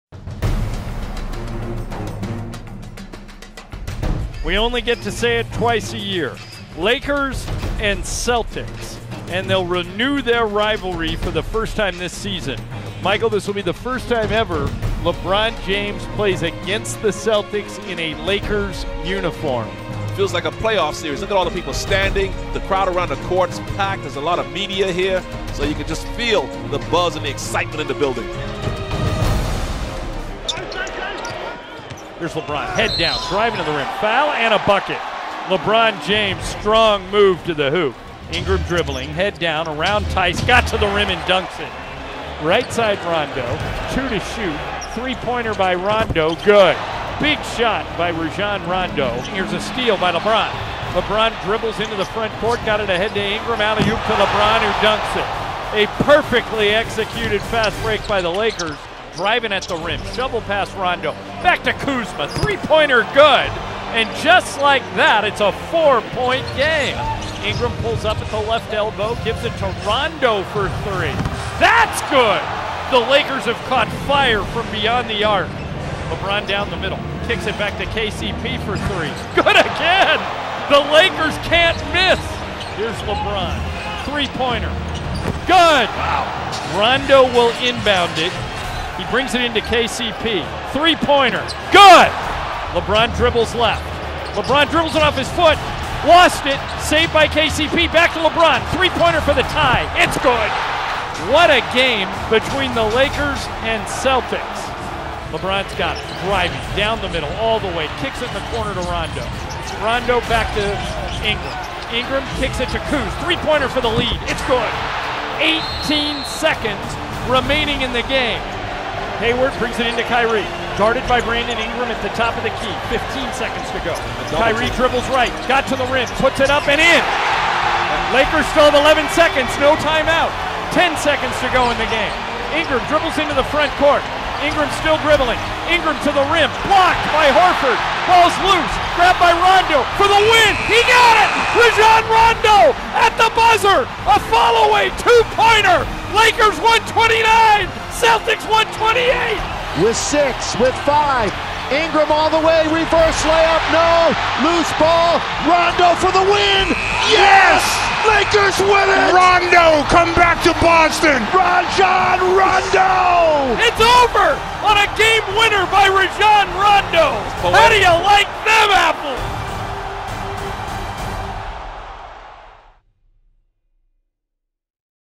Mini-Movie: Rondo Beats the Buzzer in Boston (2/7/19)
After losing the lead with 11 seconds left in the game, Rajon Rondo scooped up a loose ball and knocked down the game winning shot as time expired.